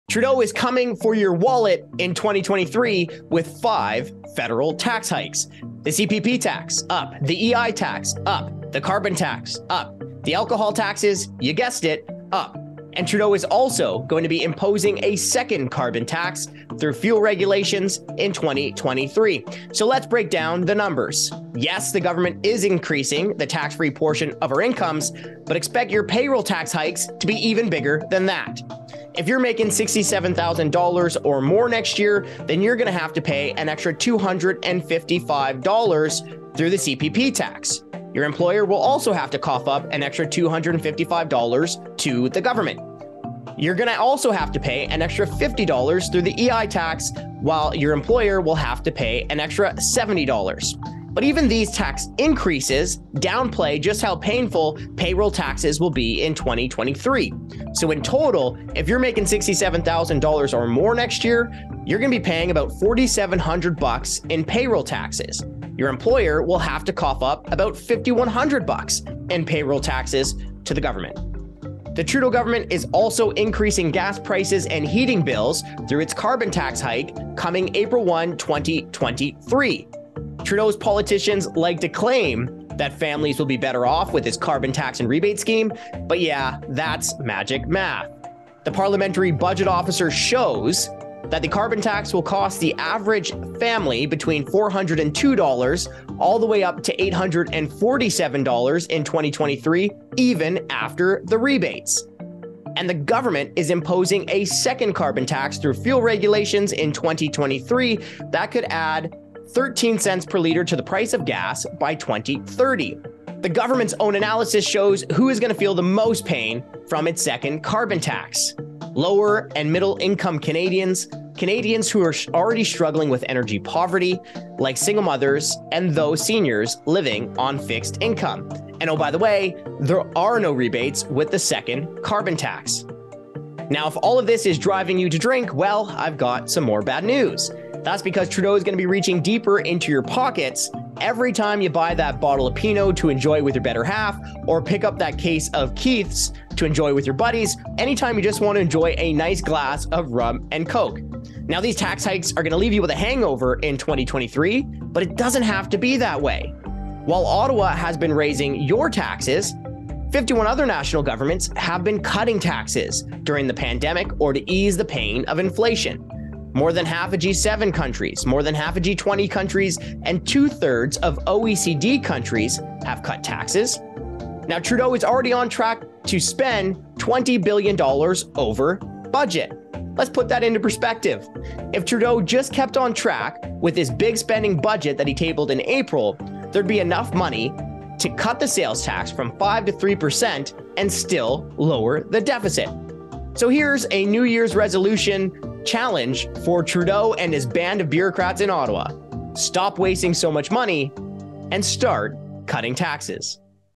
Trudeau hiking 5 taxes in 2023
There's *FIVE* federal tax hikes coming in 2023: CPP tax hike, EI tax hike, carbon tax hike, alcohol tax hike and a new second carbon tax.

Here's a New Year's resolution for Trudeau and his band of bureaucrats:

Stop wasting so much money & start cutting taxes.